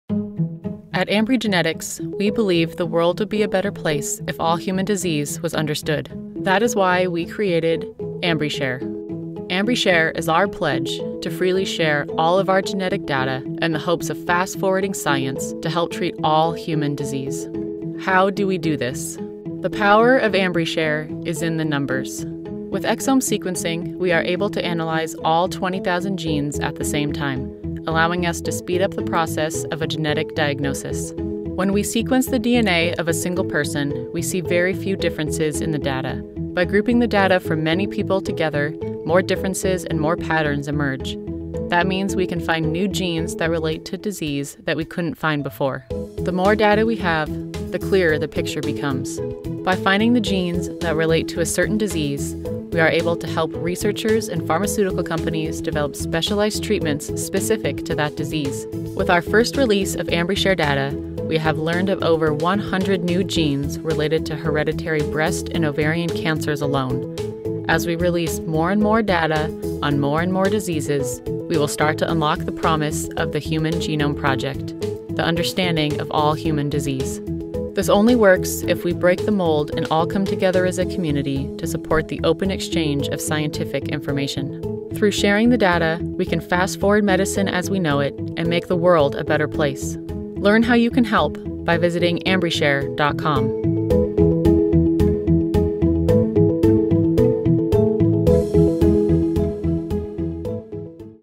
Sharing is Curing with AmbryShare™ | Ambry Genetics
At Ambry Genetics, we believe the world would be a better place if all human disease was understood.  That is why we created AmbryShare.

AmbryShare is our pledge to freely share all of our genetic data in the hope of fast forwarding science to help treat all human disease.